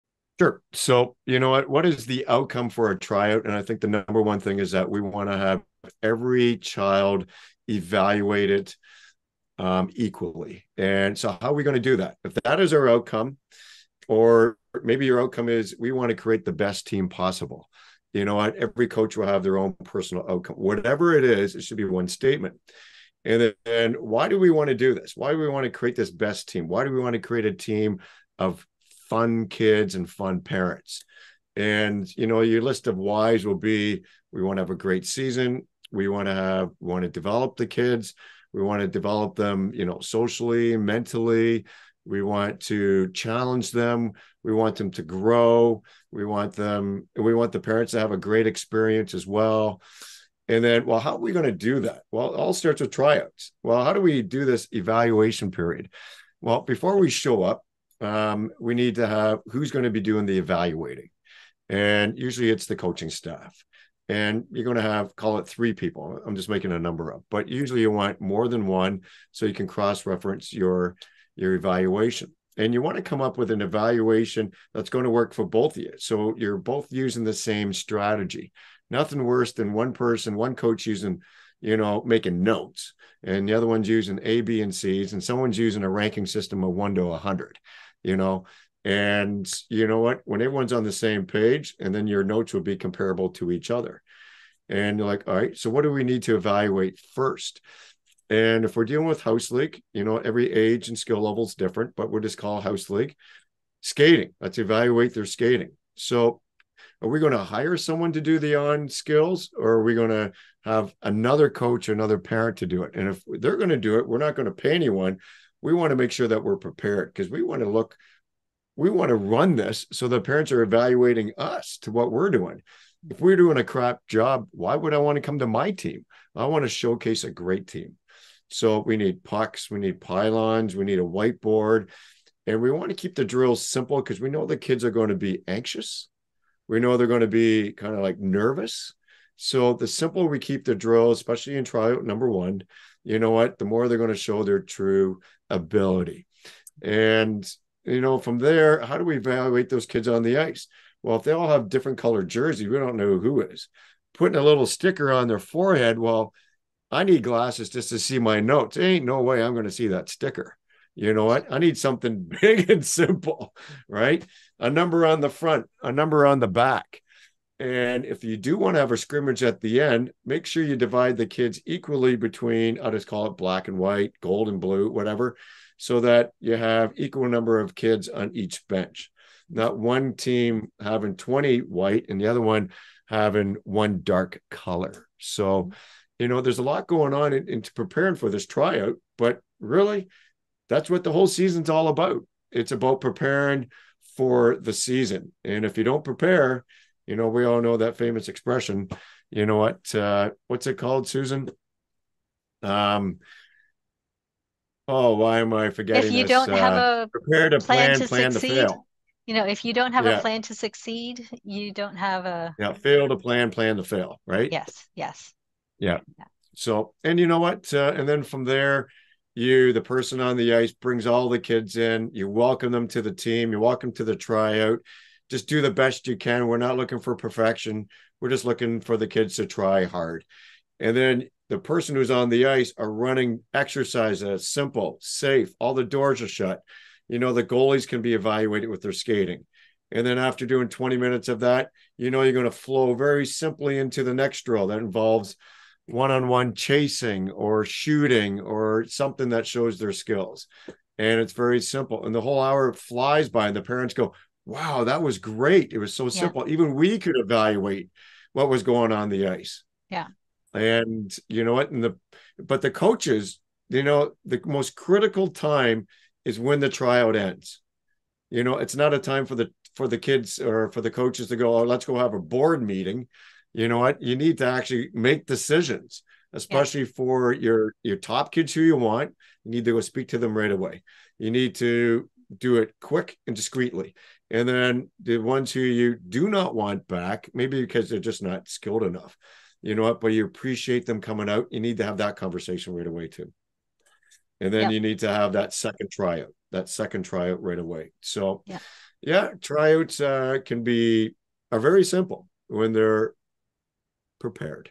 How to plan a successful hockey tryout | Coaching Call Clip
So you need to run a hockey tryout. Here's how a little planning can go a long way.